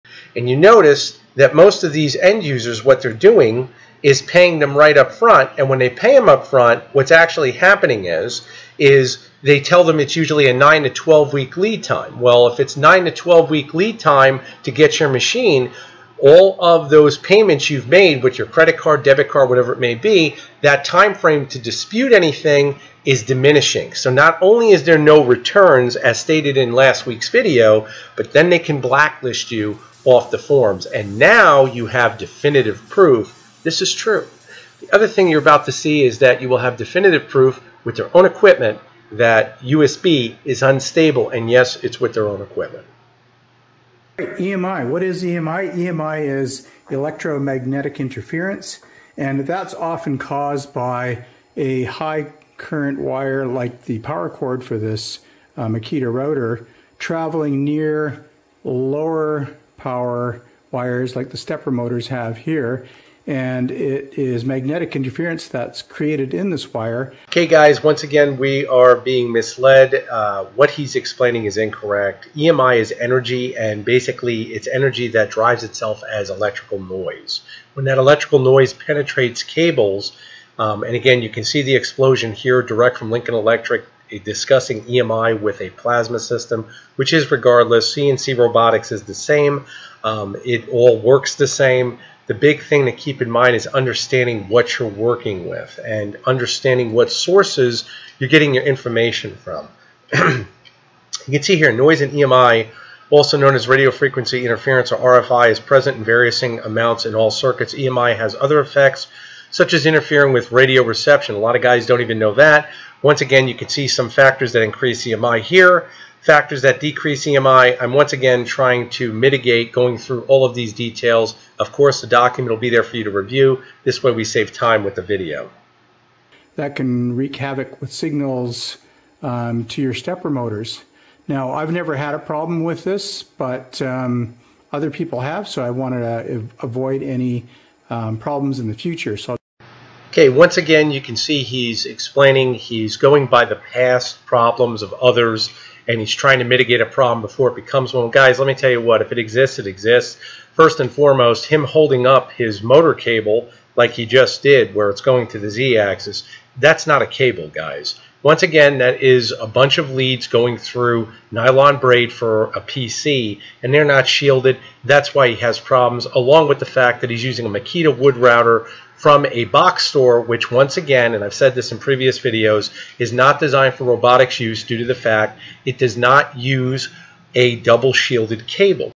Wood Router as CNC? ⚠️ Big EMI Problems! (Watch Before You Build!) 🔌🤖
🔹 "Thinking of using a wood router for your CNC build? Think again! Wood routers aren’t designed for robotics and can generate massive electromagnetic interference (EMI) due to unshielded cables. This could lead to signal loss, erratic movements, or even damage your CNC controller. Learn why this is a risky shortcut and what to use instead!"